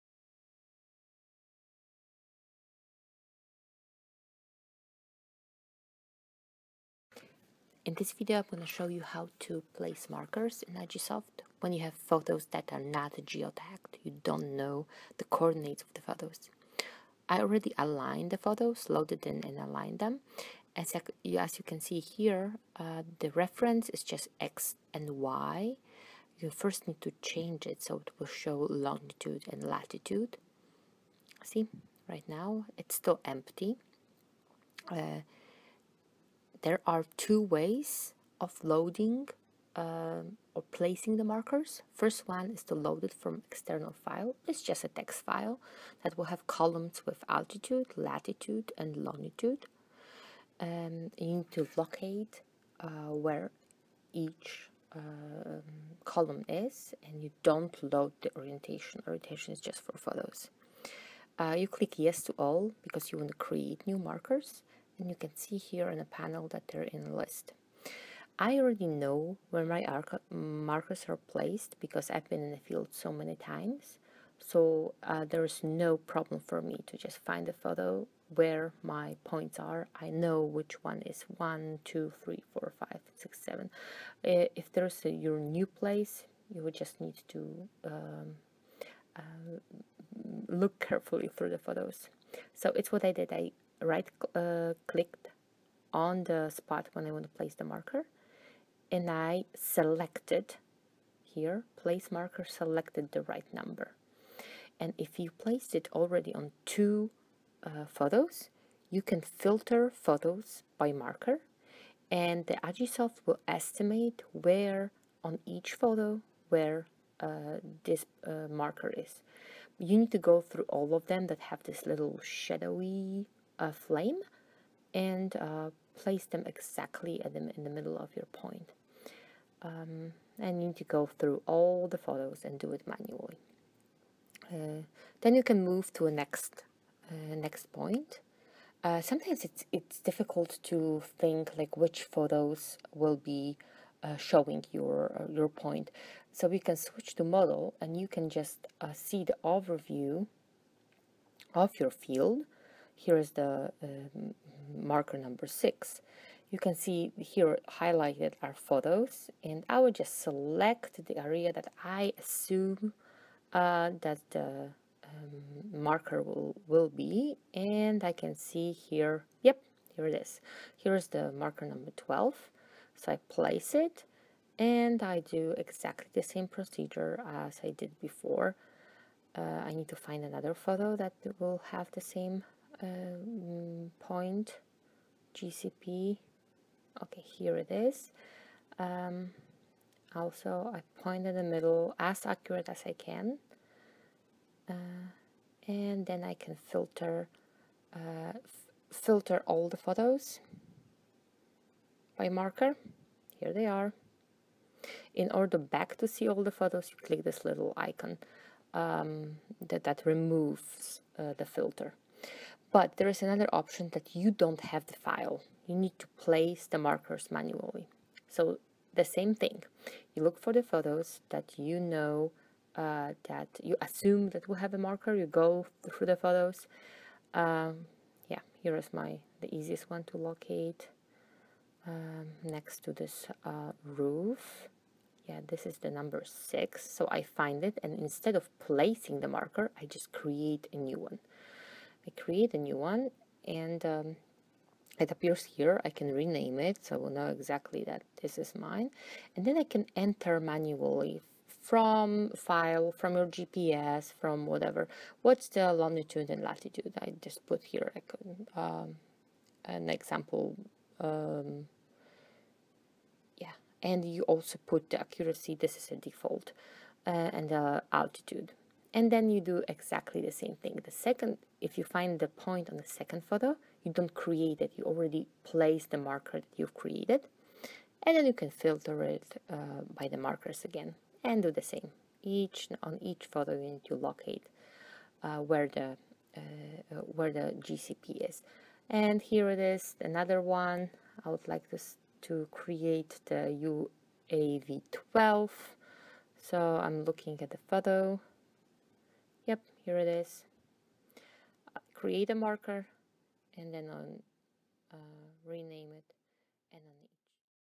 Placing Markers (Agisoft PhotoScan)
Short instruction how to place markers to georeference the model.
In this example, there is no flight log and pictures are not geotagged.
Agisoft PhotoScan version 1.2.6